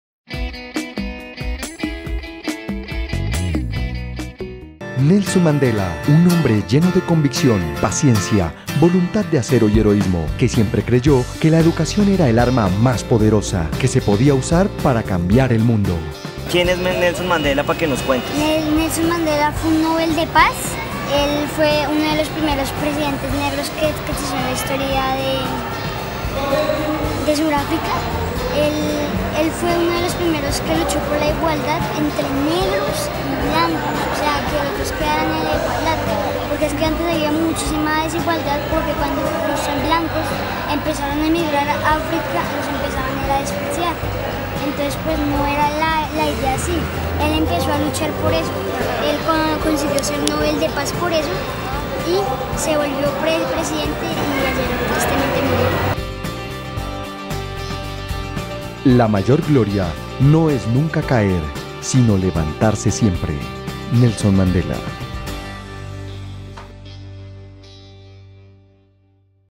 Nelson Mandela en la Personería de Bogotá
Mandela acaba de dejar este mundo mucho mejor de como lo encontró.
PERSONERÍA DE BOGOTÁ 2013